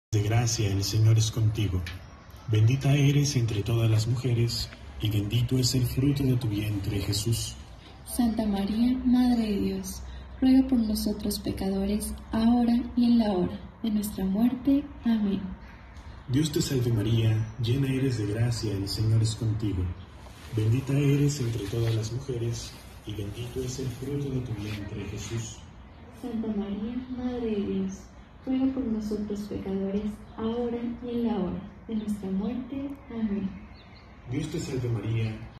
Mujeres rezan en sus casas en Hermosillo, Sonora, por Coronavirus
Así rezan 🙏 mujeres desde sus casas en fraccionamiento Palermo en Hermosillo, Sonora, en tiempos del Coronavirus 🦠